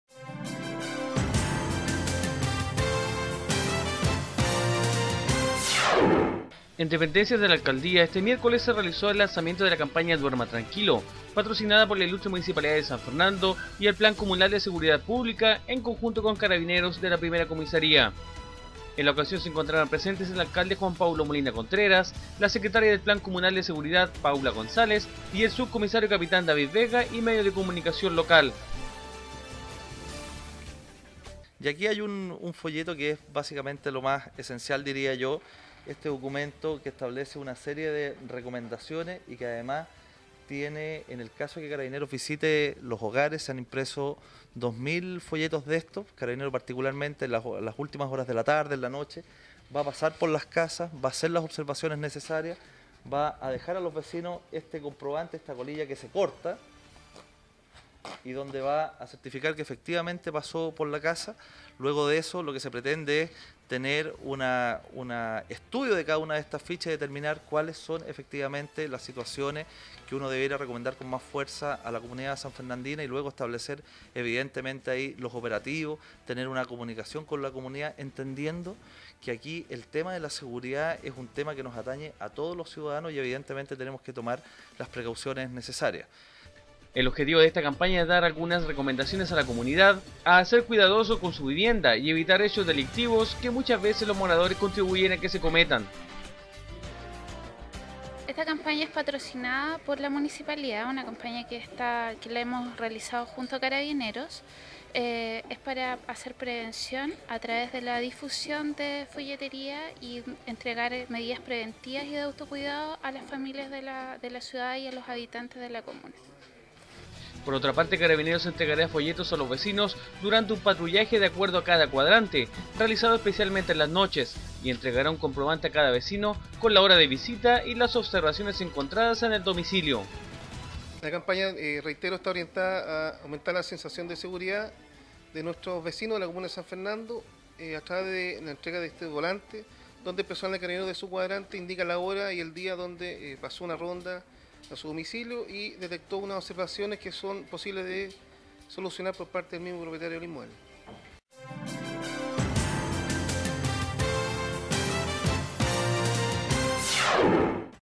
Campaña Duerma Tranquilo
Lanzamiento de campaña por Municipalidad de San Fernando y Carabineros de la 1ra. comisaria de la comuna